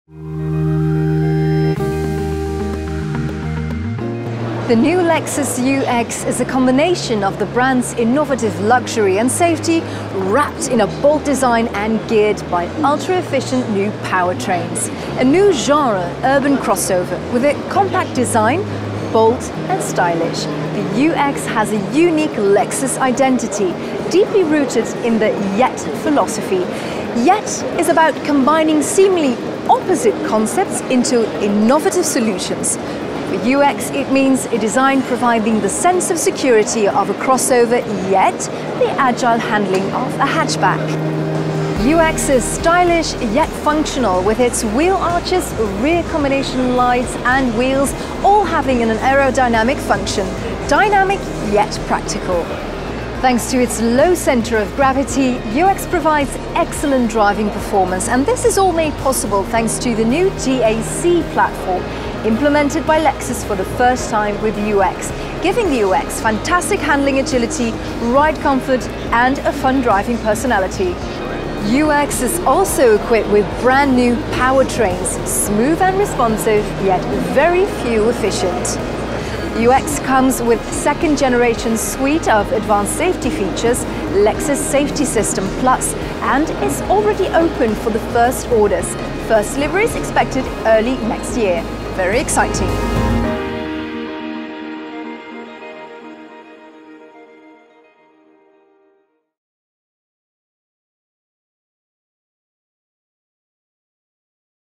The all new Lexus UX at 2018 Paris Motor Show | Lexus Europe
The new Lexus UX offers the brand’s innovative luxury and safety in a package that combines bold new design elements and ultra-efficient new powertrains.

The all-new Lexus UX challenges everything that went before. Its bold design expresses unmistakable strength. Yet its elegantly sculpted surfaces suggest a character that is both stylish and dynamic.